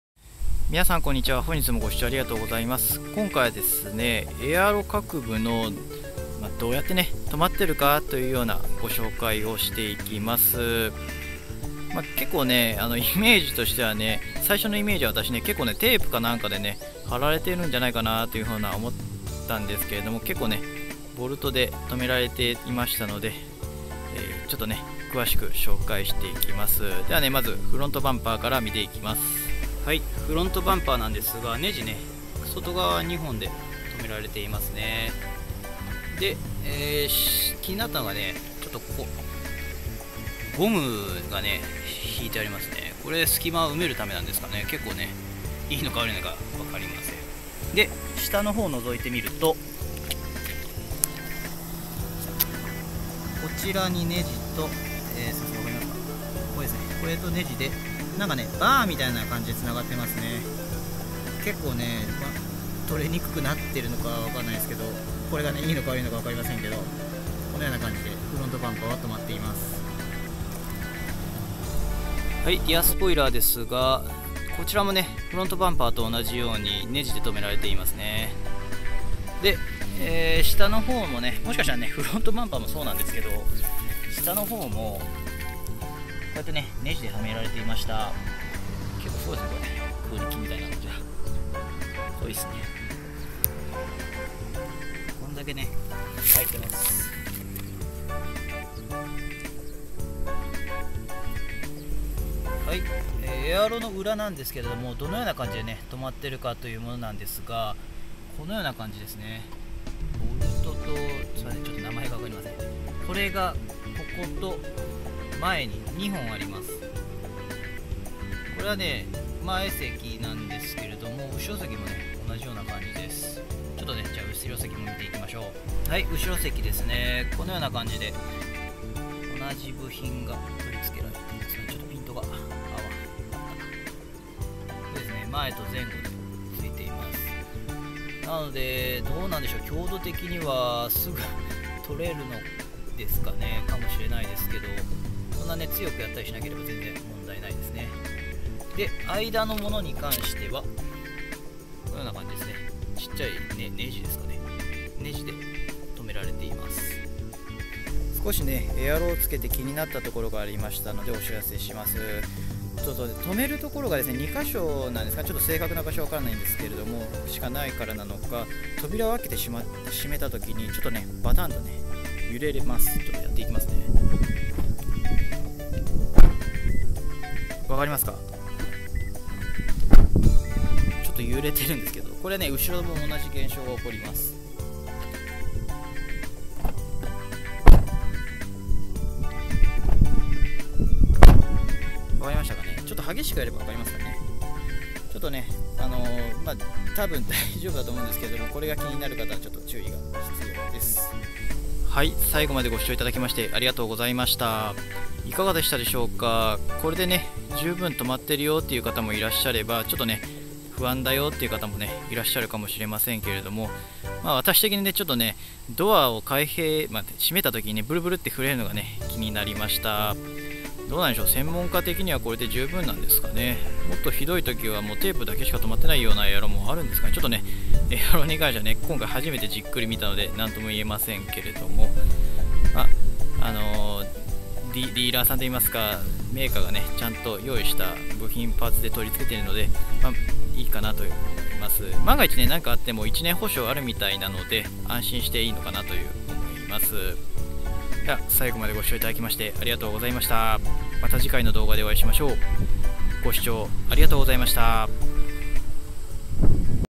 Vlog/ハリアー トムスエアロはどのように固定してあるか？
今回はハリアーに装着している、
トムスエアロがどのように固定してあるか確認していきます。

トムス関連動画